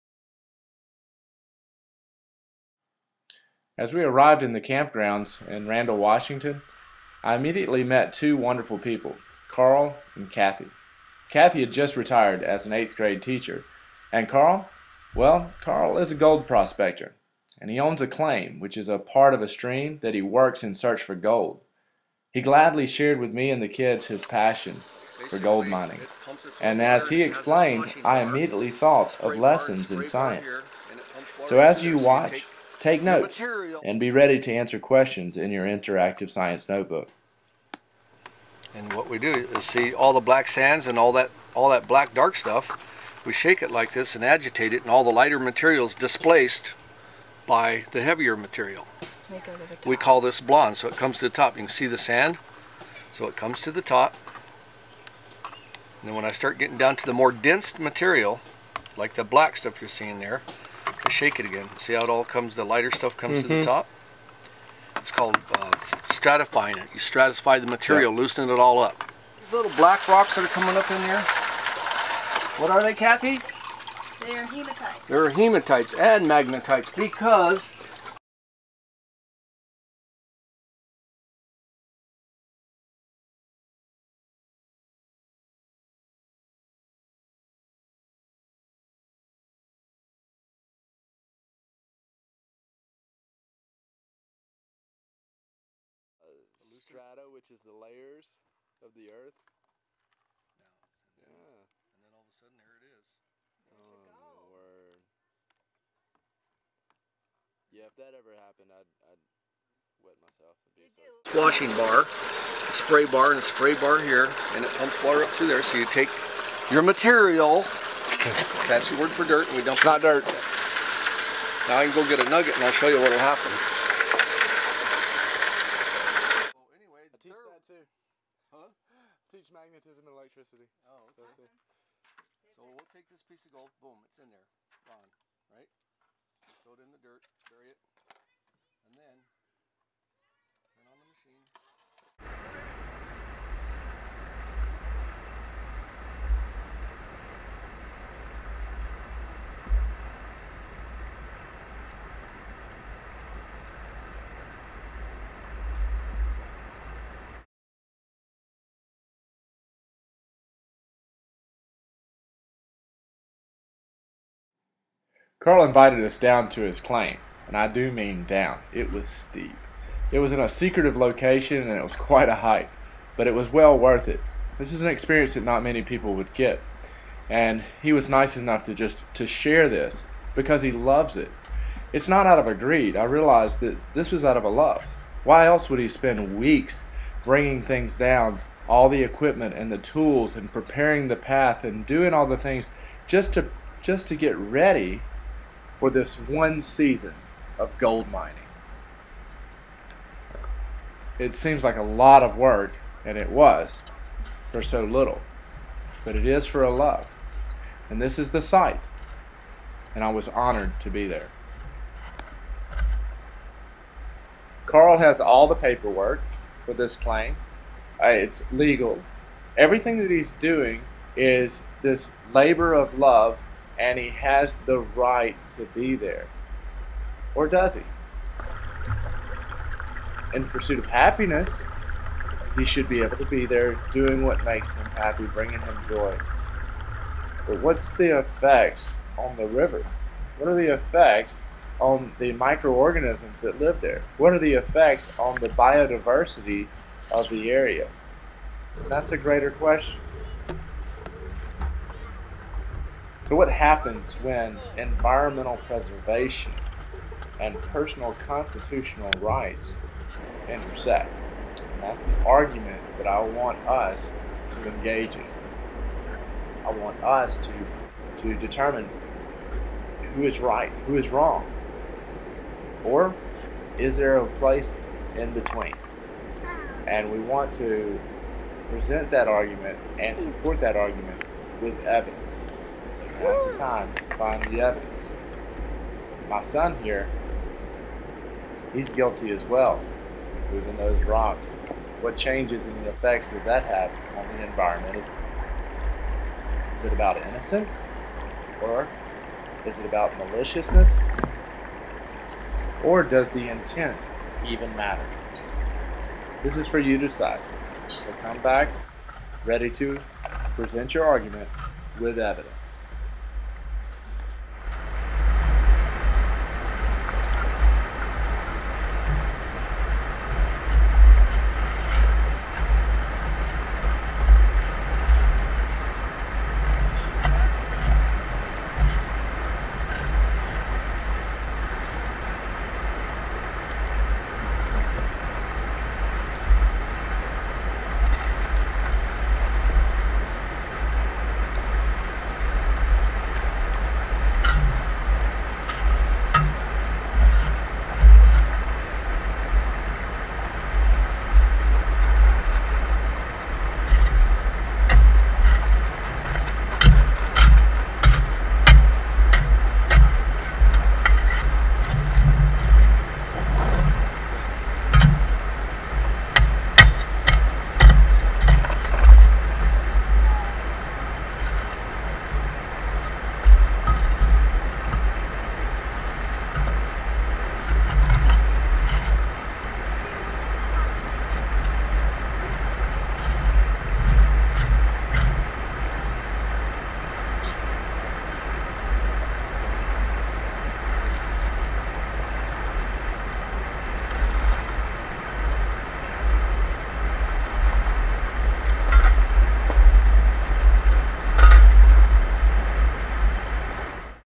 Gold Rush: Lessons in Science from the Field
As a teacher, I traveled across America to learn and create videos for my 8th grade science classes. This video has two sections that will be used, covering two subjects: Density and Environmental affects on Biodiversity. The density of gold allows Carl to separate it from the rest of the material by using the power and dynamics of the water to help it settle in the bottom. The second concept that we will explore is the environmental impact of disturbing the river bed. The material is the sediment which when disturbed causes changes to the water and the environment in which plants and animals live. The argument that I want my students to engage in is whether our personal rights should infringe on protecting the environment. The students will research and defend their claim with evidence. My hope is for them to form strong opinions based off of researched evidence but then find a middle ground, which is similar to the job of the government.